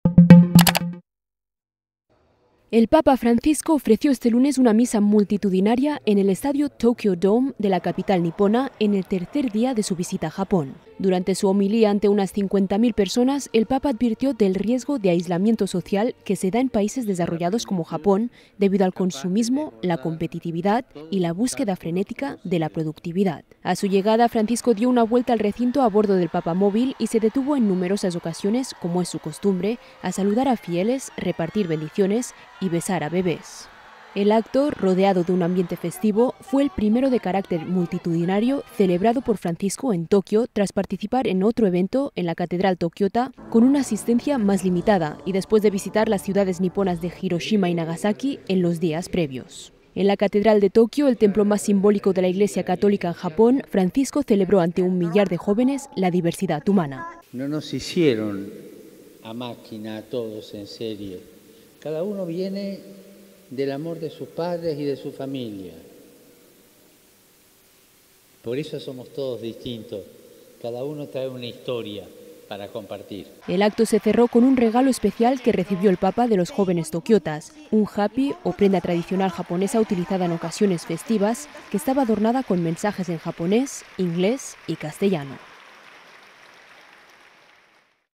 El papa alerta del "aislamiento social" en una misa multitudinaria en Tokio
Tokio (Japón), 25 nov (EFE).- (Imagen: Antonio Hermosín/Yoko Kaneko).- El papa Francisco advirtió hoy del riesgo de "aislamiento social" que se da en países desarrollados como Japón debido al "consumismo", "la competitividad" y "la búsqueda frenética de la productividad", durante una misa multitudinaria celebrada en Tokio.
Contiene imágenes cedidas por el Vaticano.